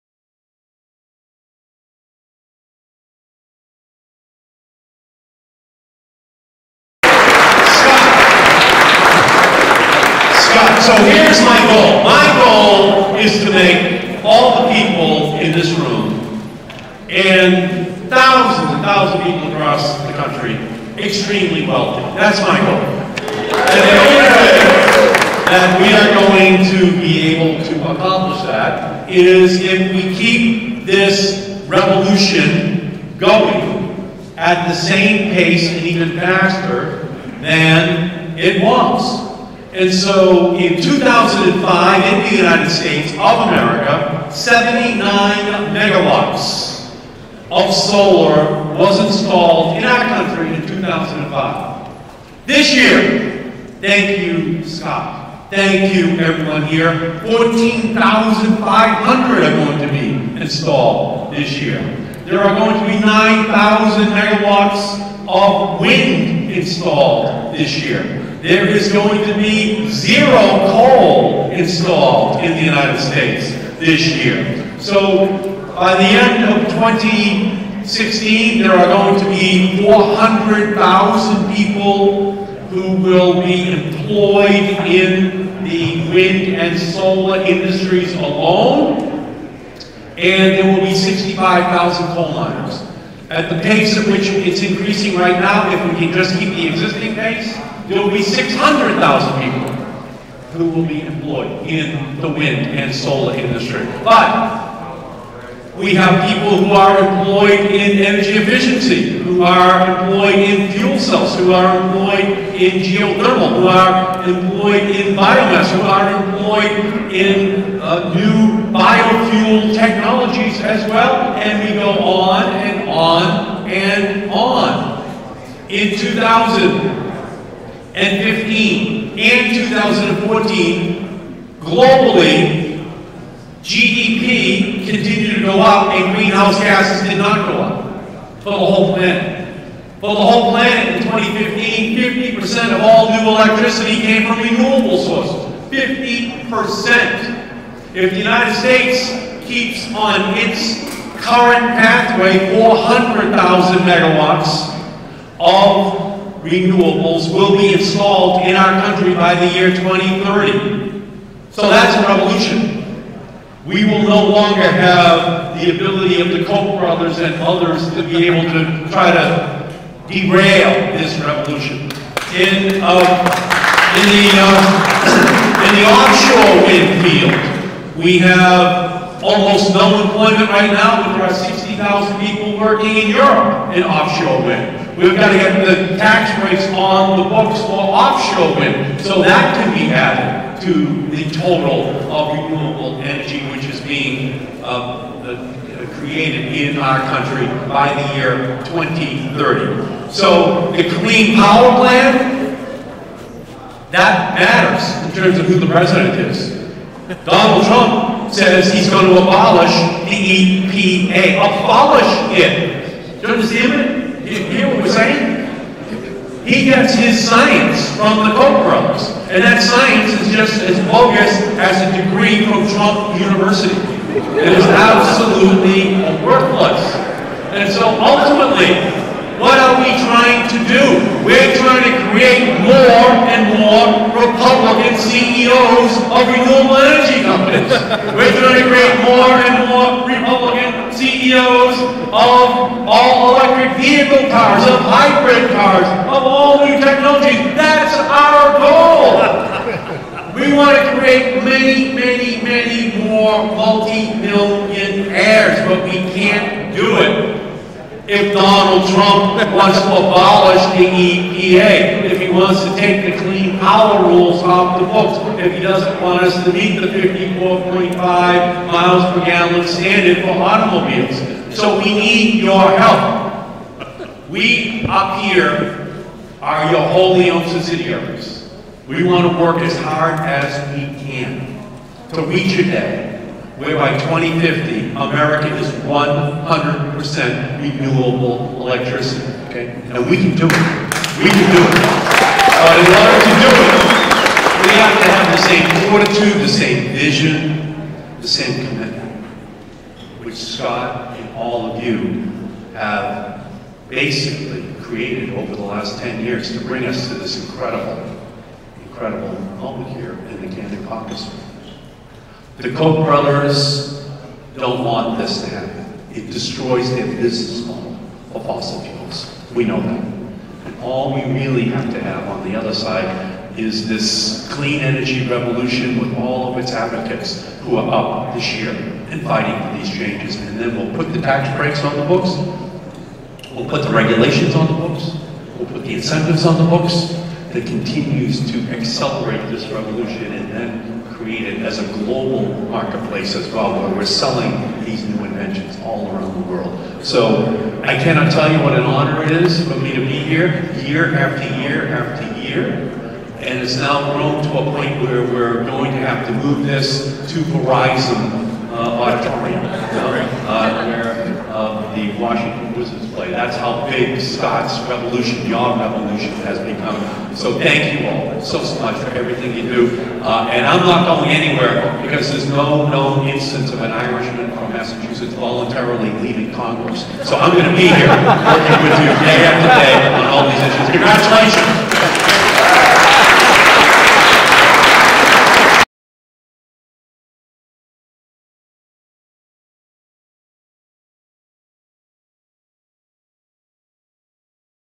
Expo 2016: Senator Ed Markey
The 19th Annual Congressional Renewable Energy and Energy Efficiency EXPO + Policy Forum brought together 50 businesses, trade associations, and government agencies to showcase renewable energy and energy efficiency technologies. In every state across the country, these technologies are having a significant impact in business development and job creation in the manufacturing, transmission, power, transportation, and building sectors.